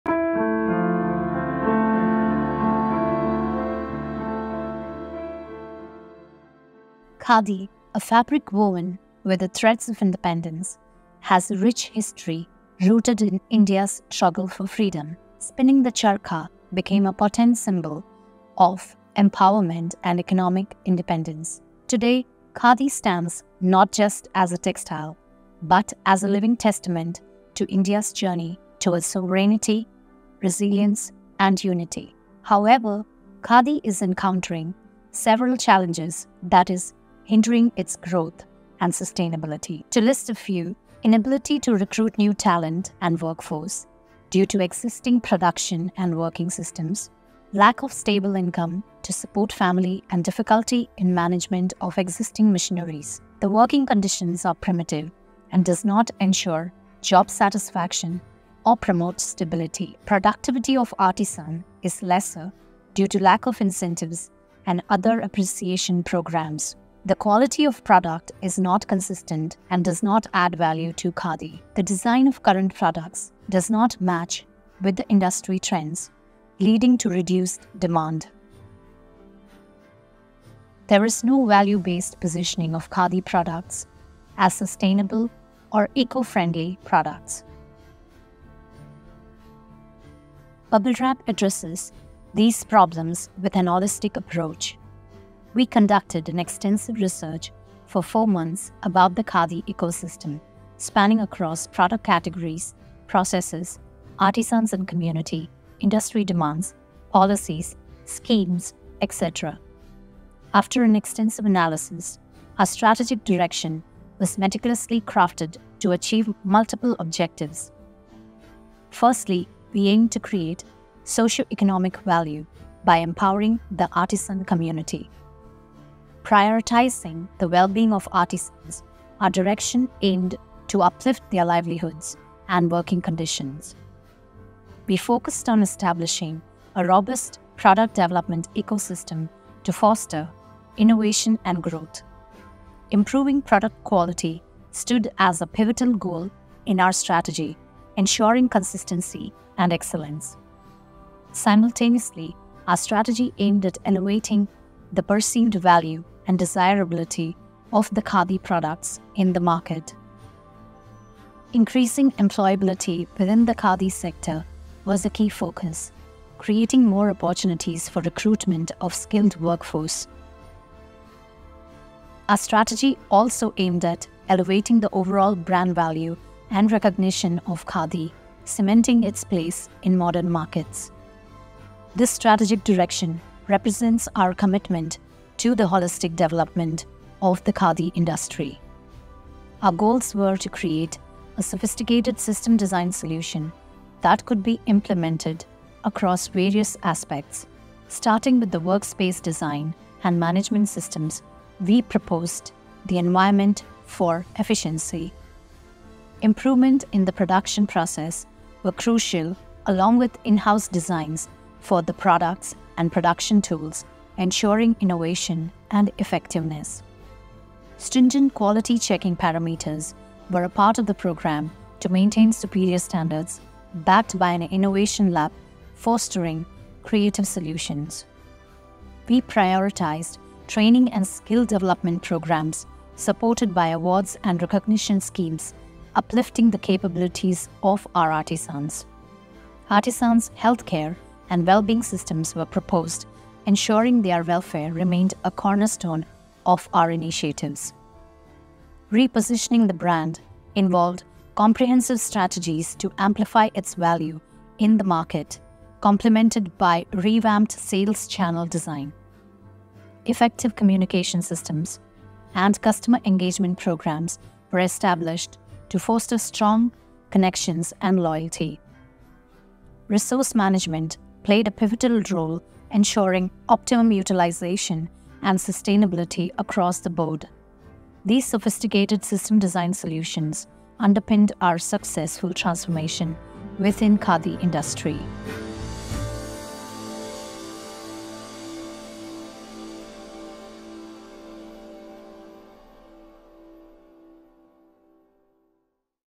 Khadi Holistic Development Project | Case Study Video | Bblewrap
Khadi has a rich history rooted in India's struggle for freedom. However, today it is facing unfair competition from  machine-produced textiles that are cheaper and readily available. This in turn has affected the income of Khadi weavers.
Bblewrap proposed to address this problem with a holistic approach, involving social innovative activities and directing them to good sales. As per our proposal, the conventional 1.5-year production process will be swiftly condensed to just three months, showcasing a shift in our commitment to efficiency and innovation. With this strategic approach we would be able to diminish the gap between quality and productivity.

Production: Media Team Bblewrap